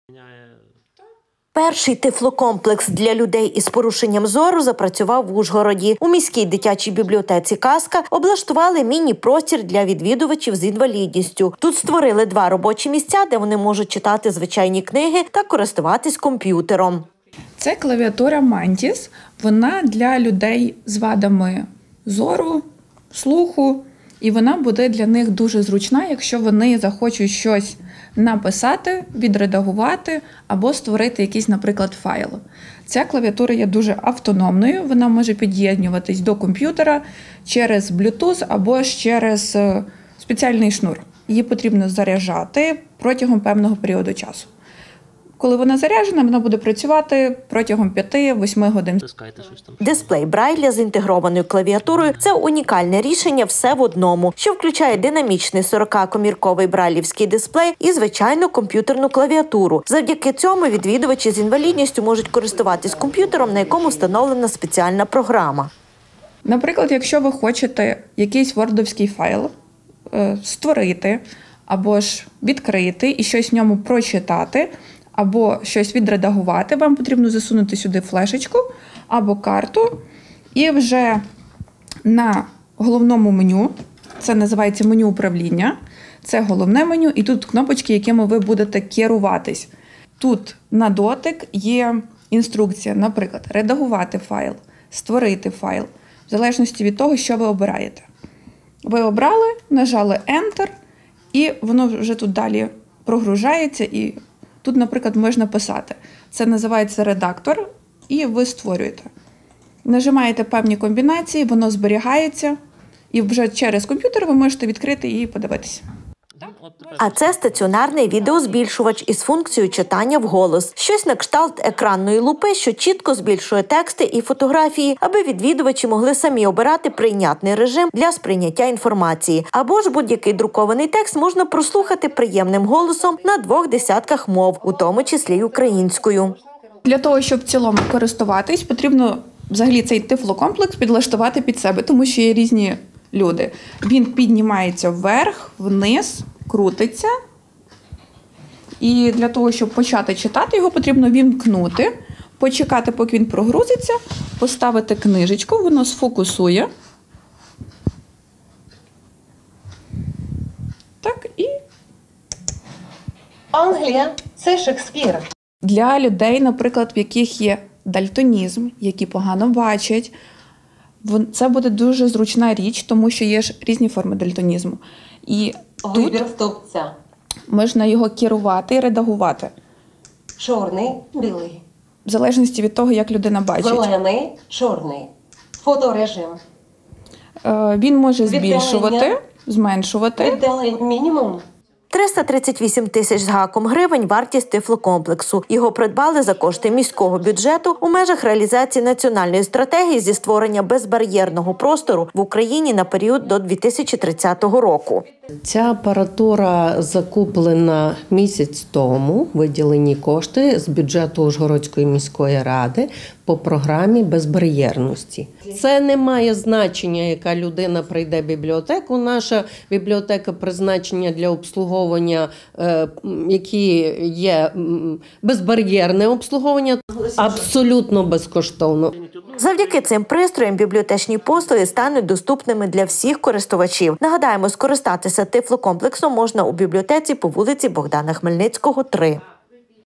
Запрацював тифлокомплекс
Перший тифлокомплекс для людей із порушеннями зору запрацював в Ужгороді. У міській дитячій бібліотеці «Казка» облаштували міні-простір для відвідувачів з інвалідністю.  Тут створили два робочі місця, де вони можуть читати звичайні книги та користуватись компʼютером. Дисплей Брайля з інтегрованою клавіатурою – це унікальне рішення  «все в одному», що включає динамічний 40-комірковий брайлівський дисплей і звичайну комп’ютерну клавіатуру. Завдяки ньому відвідувачі із інвалідністю можуть користуватись комп’ютером, на якому встановлена спеціальна програма. А це - стаціонарний відеозбільшувач із функцією читання вголос. Щось на кшталт екранної лупи,  що чітко  збільшує тексти і фотографії, аби відвідувачі могли самі обрати прийнятний режим для сприйняття інформації.  Або ж будь-який друкований текст  можна прослухати  приємним голосом на 2 десятках мов, у тому числі й українською.    338 тисяч з гаком вартість тифлокомплексу. Його придбали за кошти міського бюджету у межах реалізації Національної стратегії із створення безбарʼєрного простору в Україні на період до 2030 року .  Завдяки цим пристроям бібліотечні послуги стануть доступними для всіх користувачів. Нагадаємо, скористатись тифлокомплексом можна у бібліотеці по вулиці  Богдана Хмельницького, 3.